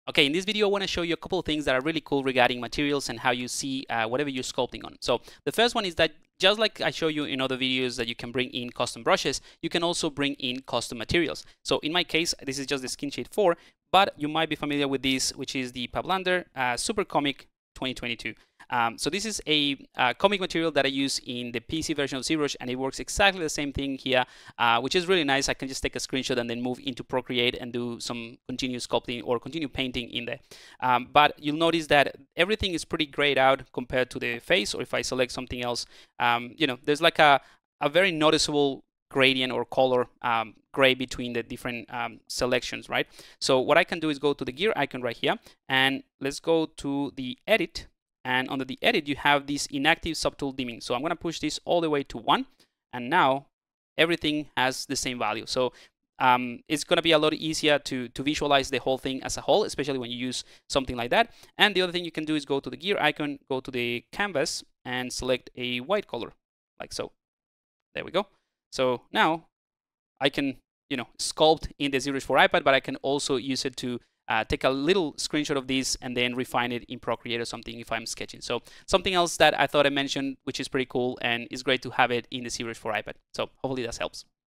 ZBrush for ipad quick tips #09 - Custom MatCaps and subtool dimming
Get your favourite MatCap materials from ZBrush into the iPad! here is a quick example of one of my Comic style materials used on the iPad.